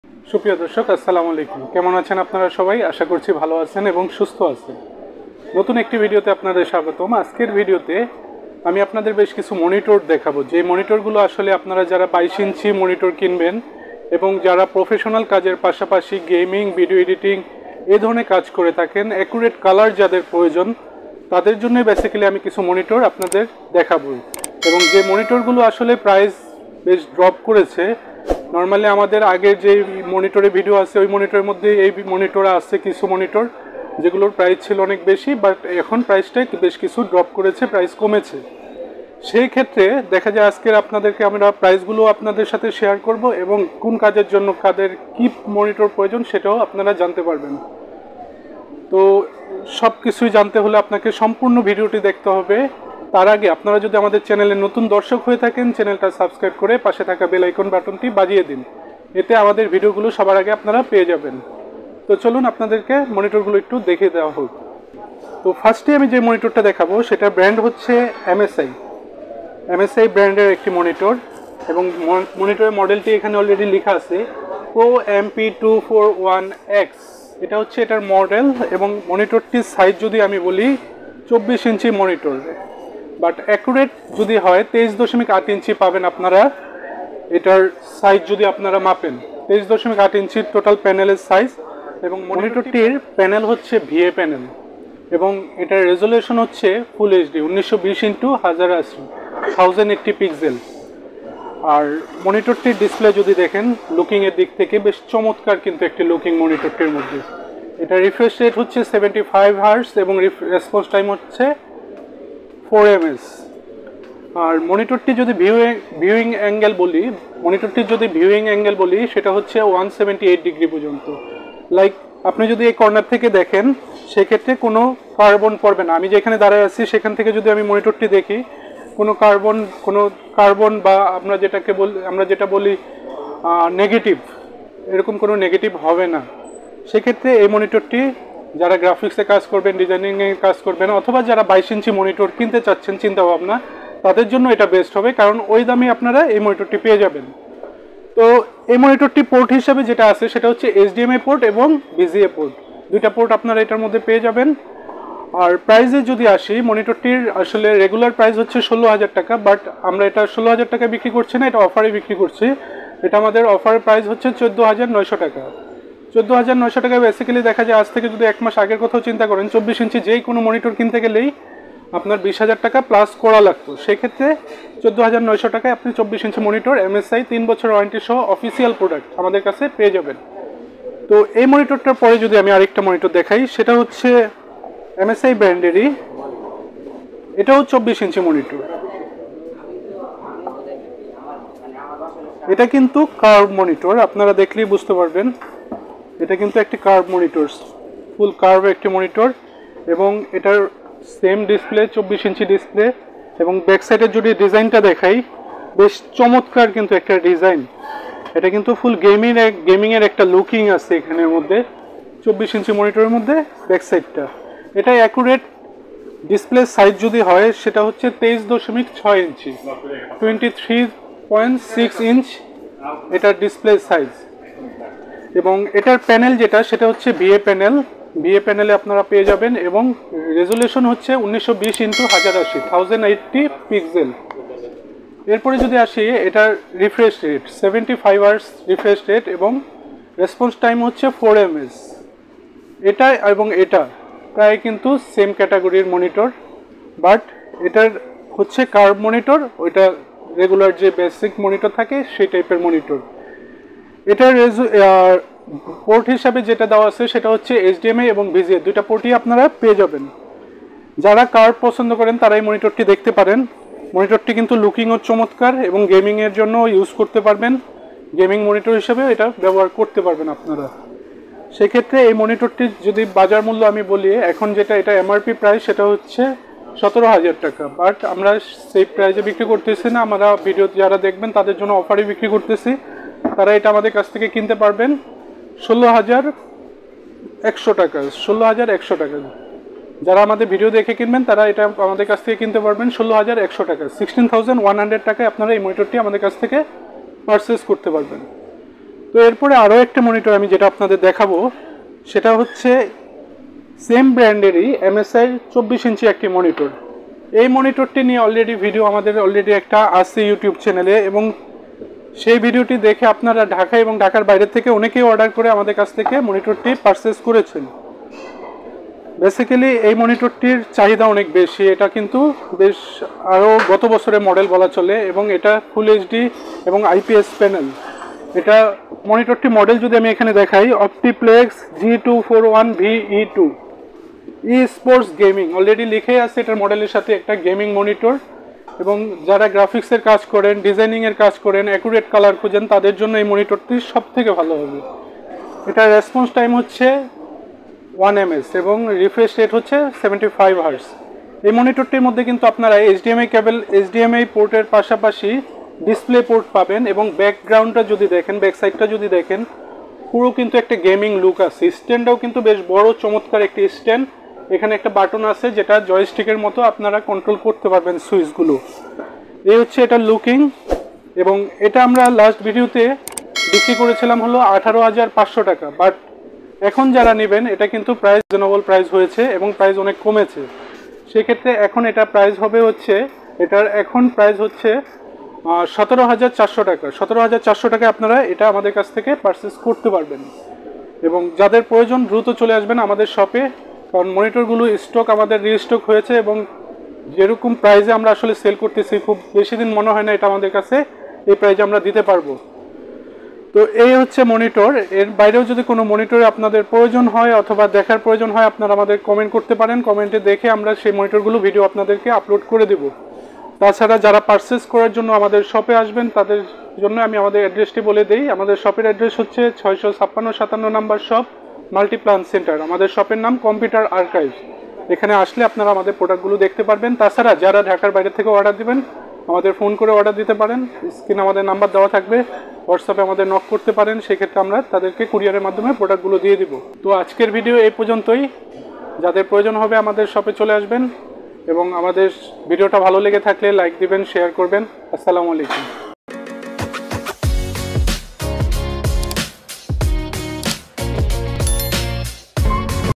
দাম কমলো নতুন মনিটরের!! | MSi brand new monitor update price in Bangladesh | Computer Archives
Monitor detail

MSI PRO MP241X 23.8" FHD Monitor
MSI PRO MP242C 23.6" FHD Curved Monitor with Built-in Speakers
MSI Optix G241V E2 24" FHD FreeSync IPS Esports Gaming Monitor

Computer  Archives

🏦ADDRESS 🏦
Computer City Center (Multiplan Center)
New Elephant Road, Dhaka-1205